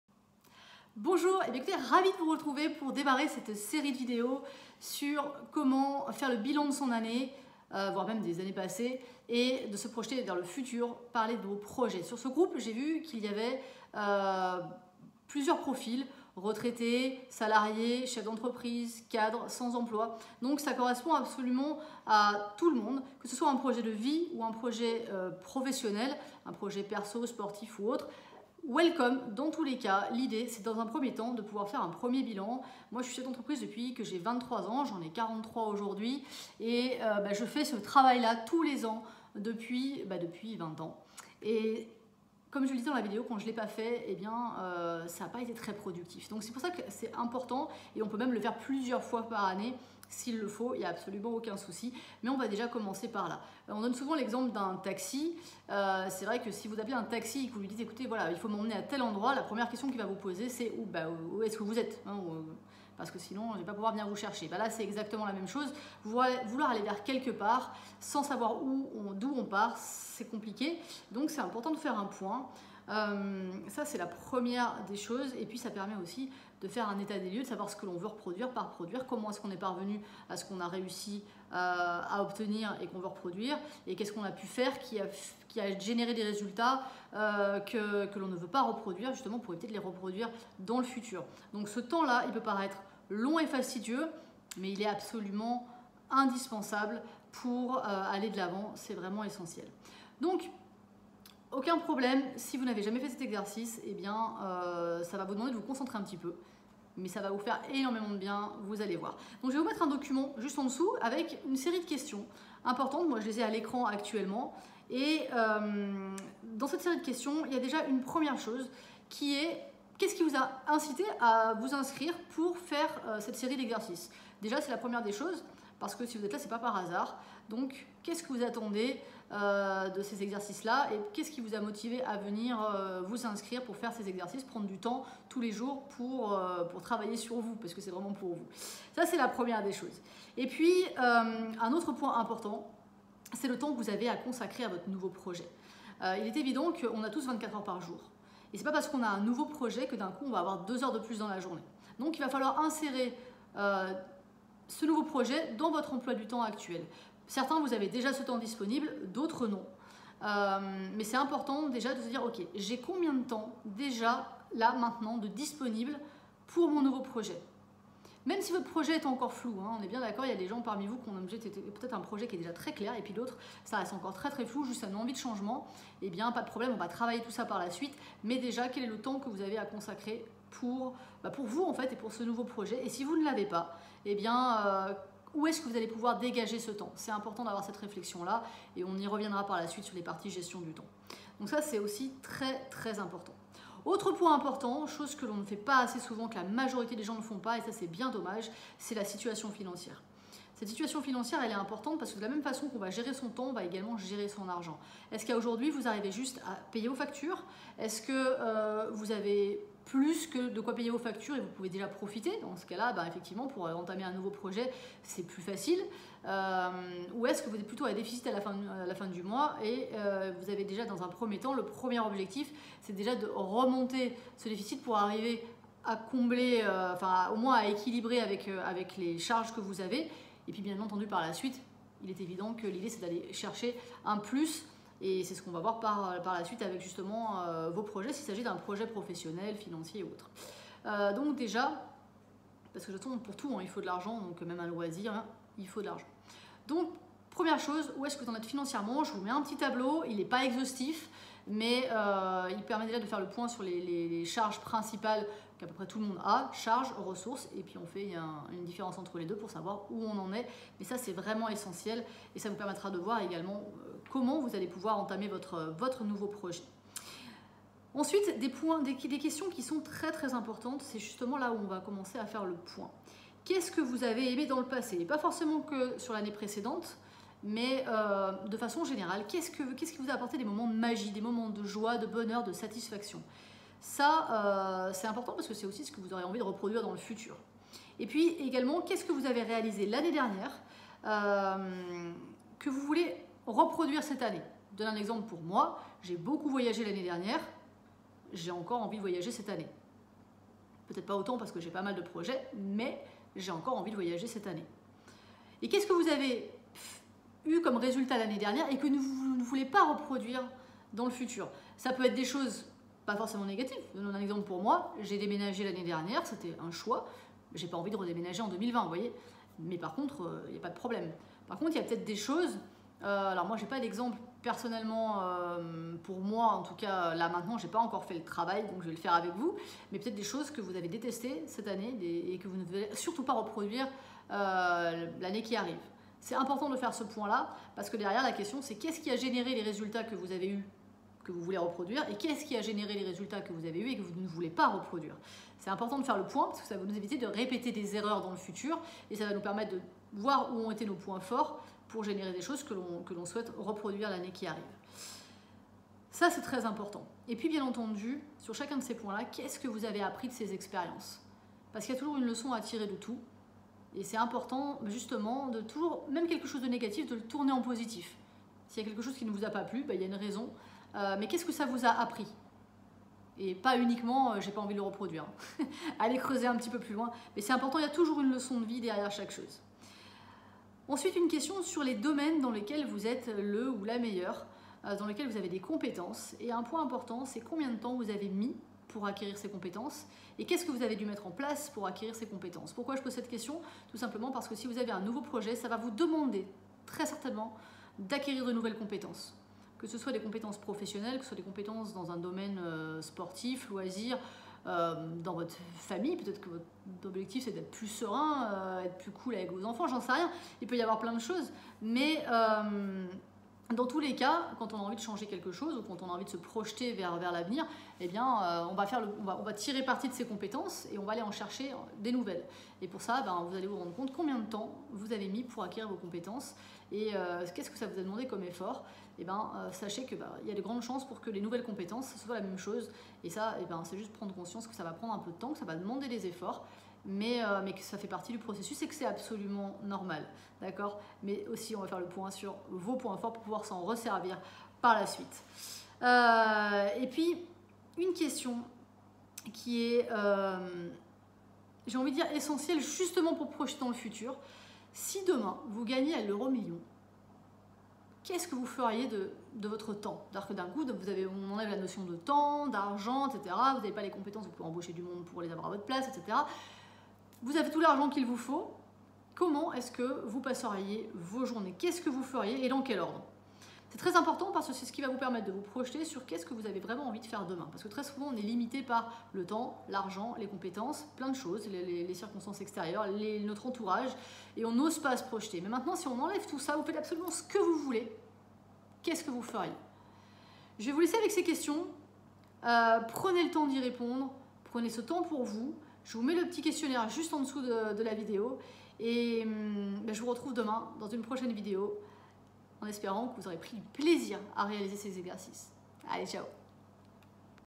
Jour 1: le Bilan
Aujourd'hui nous allons traiter d'un des sujets les plus importants de cette série, à savoir... le bilan de l'année écoulée, et votre situation actuelle.

Étape indispensable pour envisager des projets couronnés de succès cette année et les années à venir.

Prenez ce temps pour vous, prenez le comme un cadeau que vous vous faites, et pas seulement pour vous, mais également pour vos proches, car votre réussite et votre bonheur impactent aussi votre entourage ;)